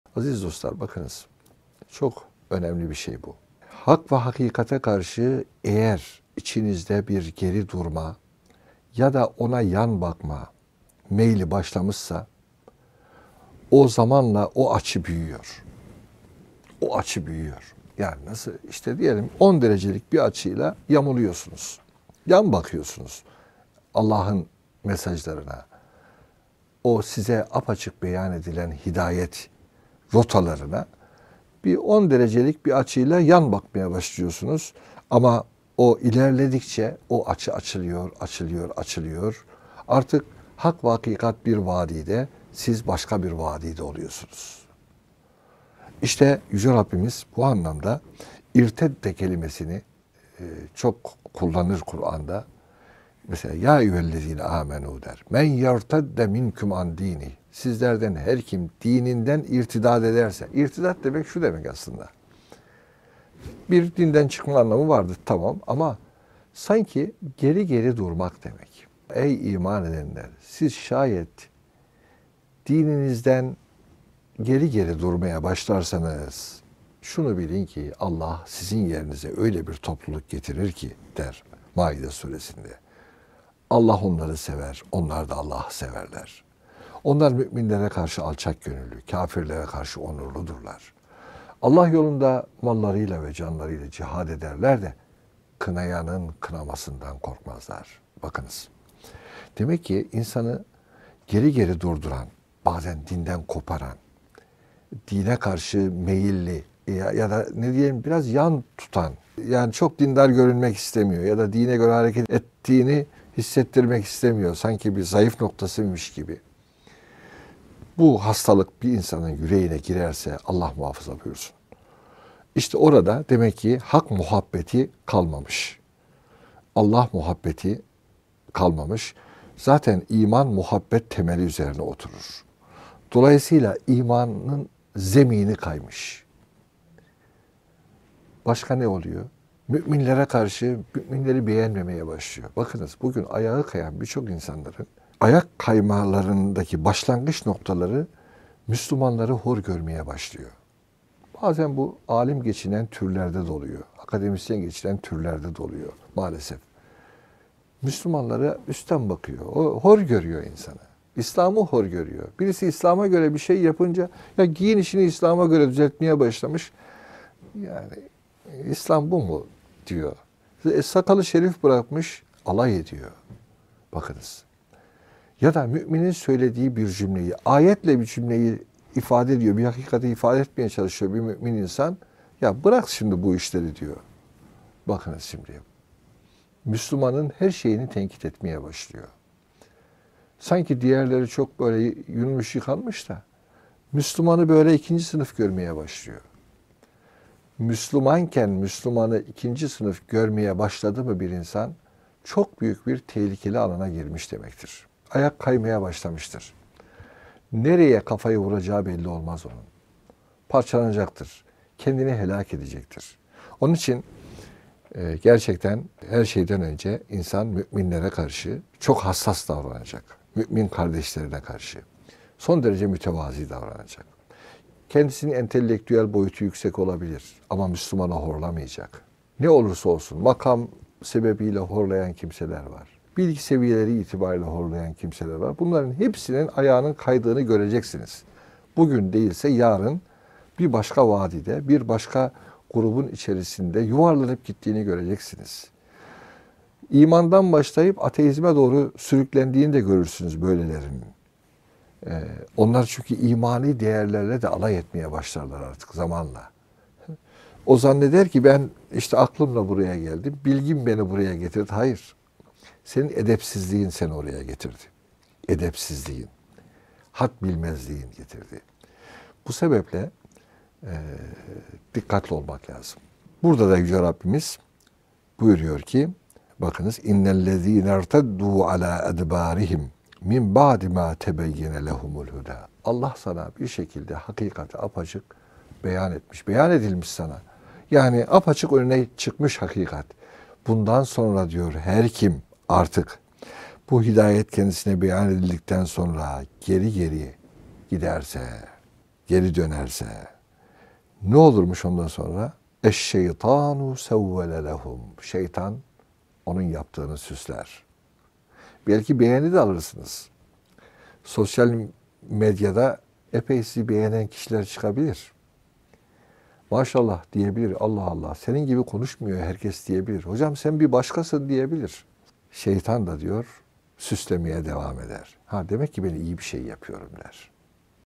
Ayağı kayan, deizme veya ateizme kayan bir çok insanın başlangıç noktası şudur... - Adem Ergül
Adem Ergül ile Tefsir Dersleri.